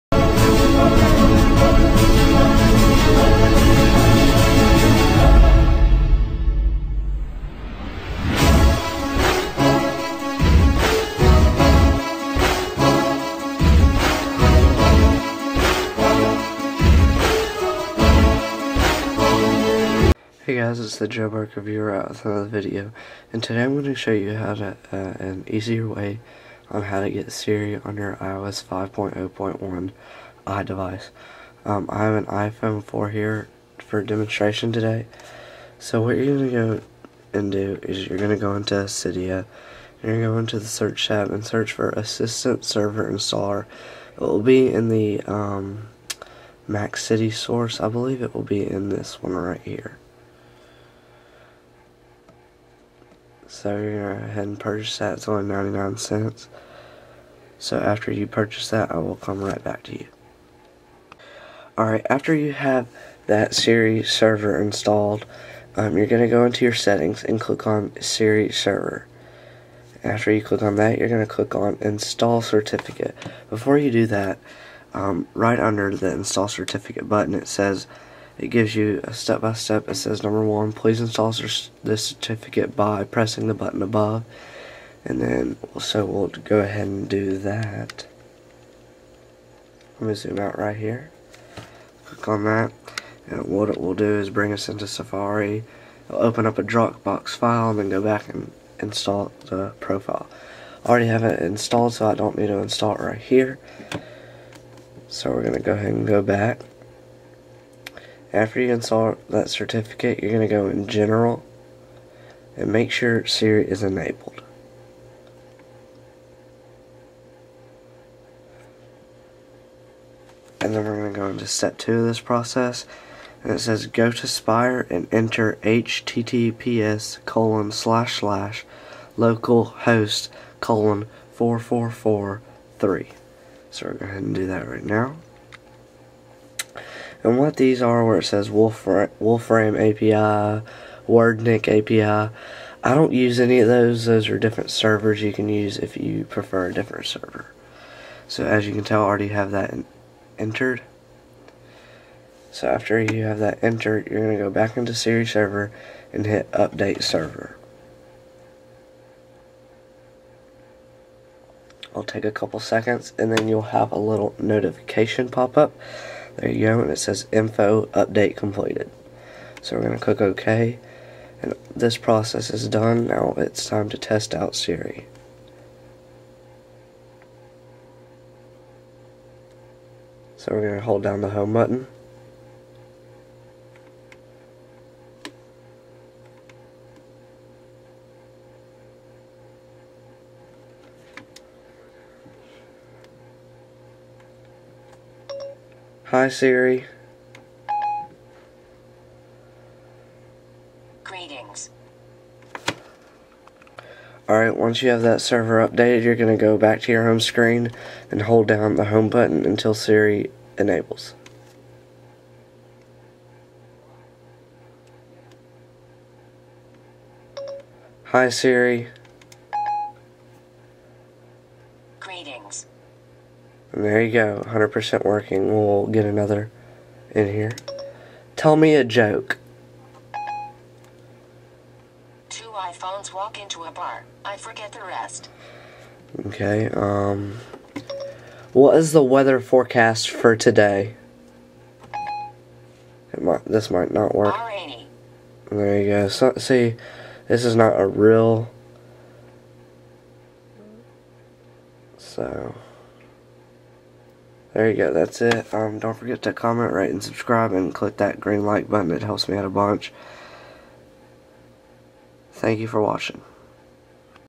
Easier Way To Get Siri 100% Working on iOS 5.0.1 on Older Devices
Name: AssistantServer Installer
Description: A siri server for non 4S devices.
Price: $0.99
Repo: MacCiti
Developer: sygex